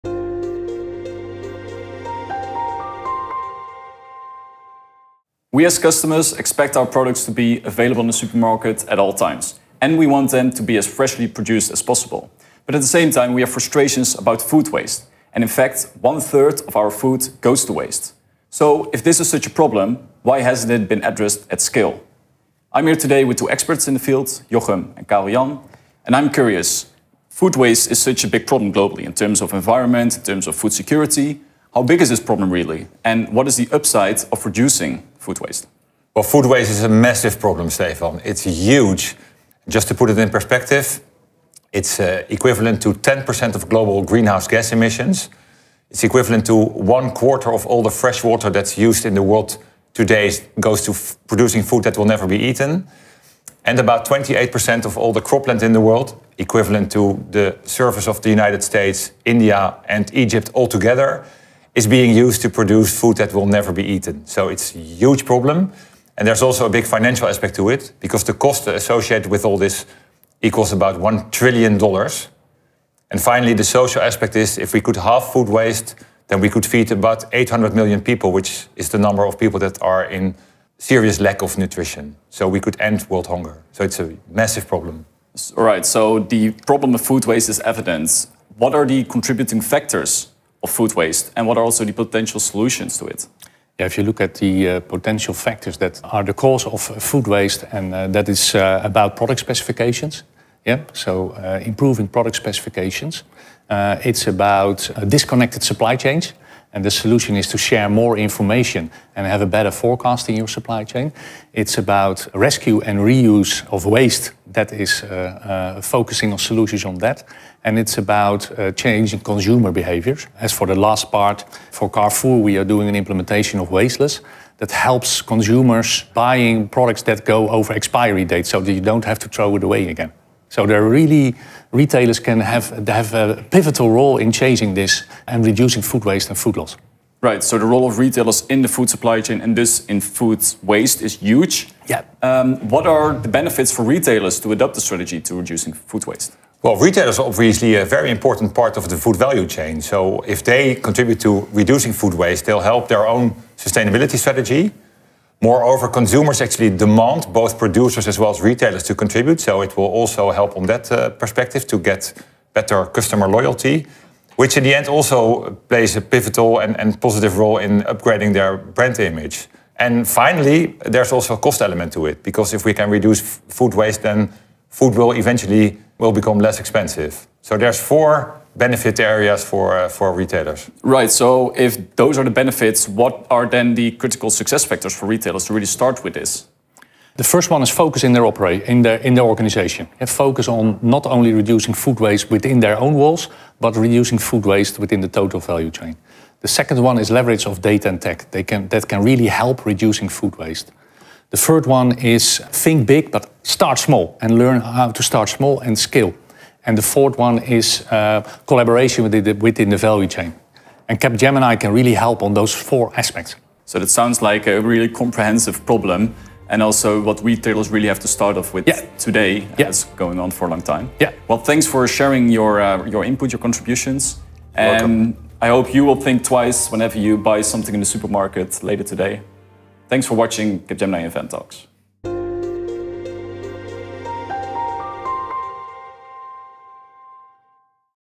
How Retailers can tackle Food Waste
In this video we tackle the pressing issue of food waste and its impact on our environment, economy, and society. Join us as industry experts discuss the current state of food waste, the role of retailers in waste reduction, and the importance of innovation and technology in streamlining supply chains.

Discover actionable insights and learn how you can make a difference in building a more sustainable future. Do you want to be part of the change?